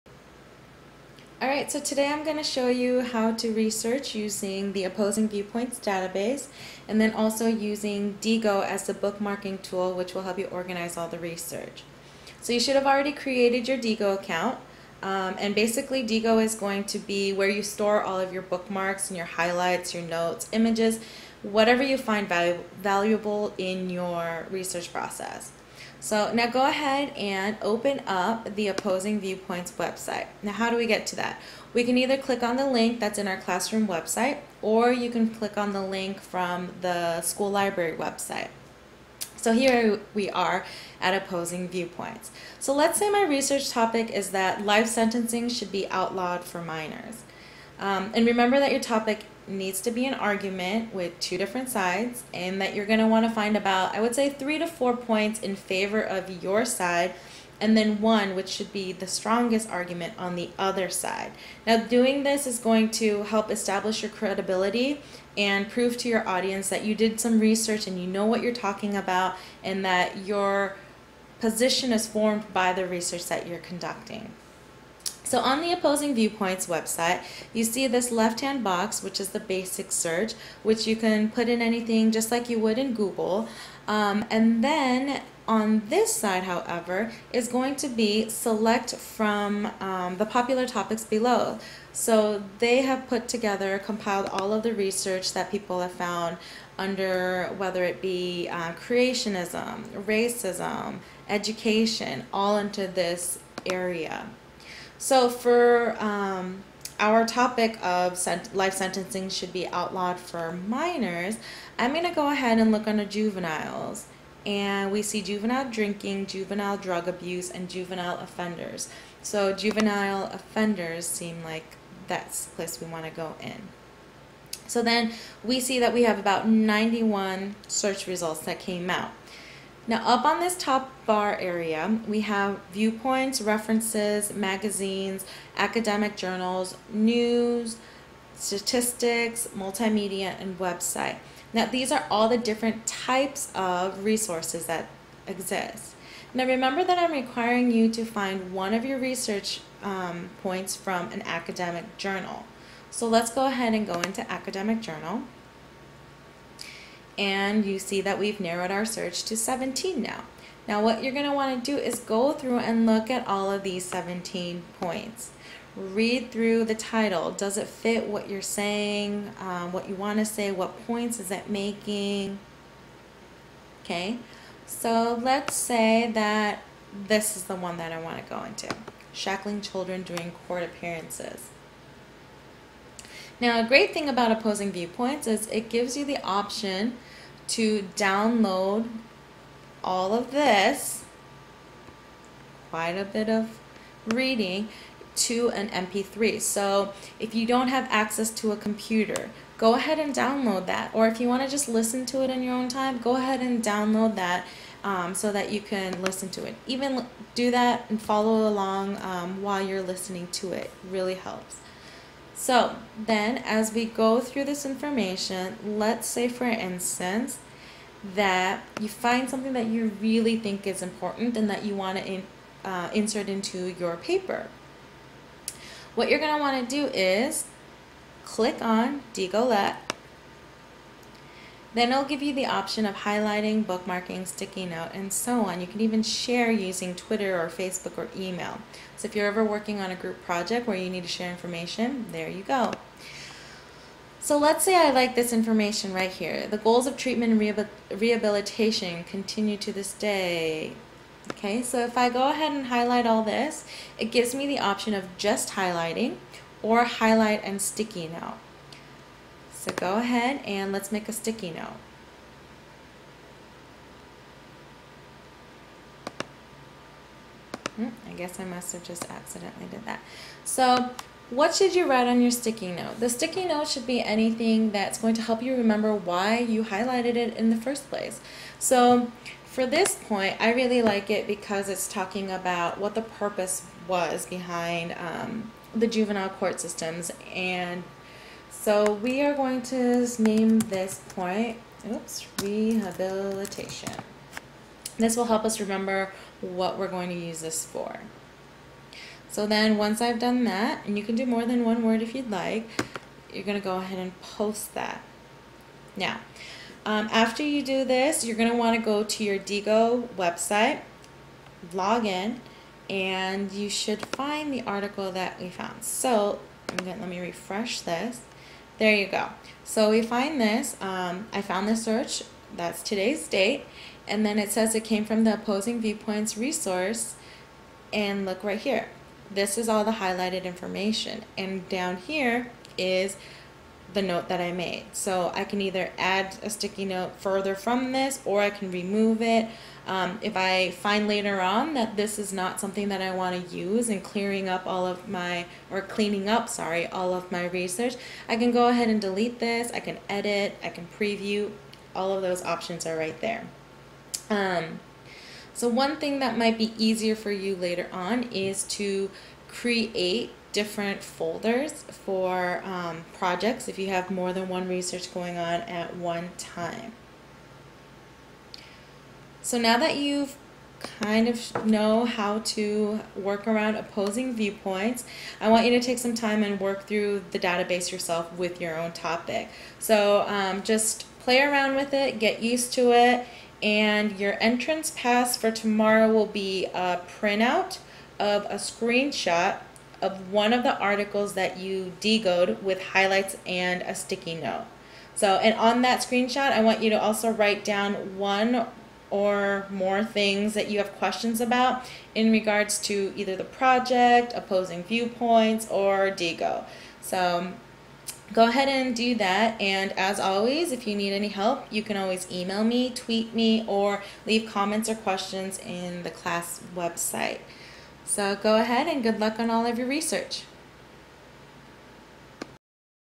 opposing viewpoints and diigo screencast
screencast of opposing viewpoints database and diigo bookmarking tool